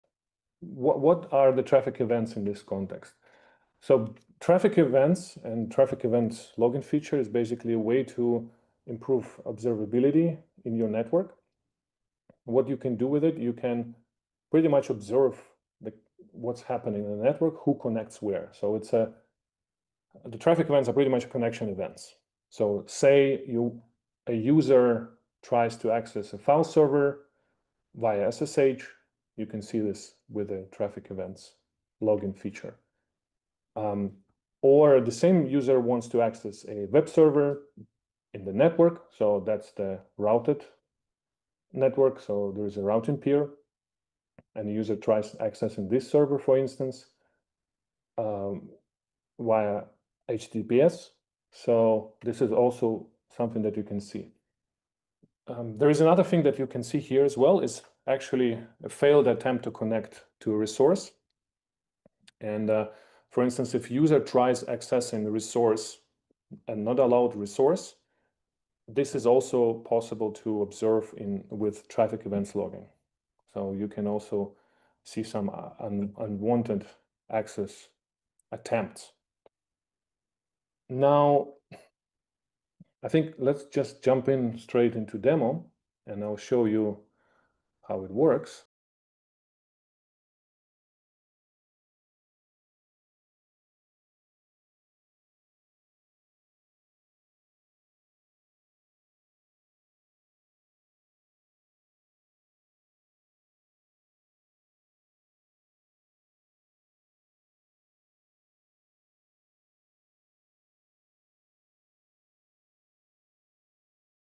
NetBird Traffic Events Logging for Private Network Observability
NetBird is an open-source zero-trust network security platform that makes secure private networking and remote access ridiculously simple for everyone—from home users to large enterprises.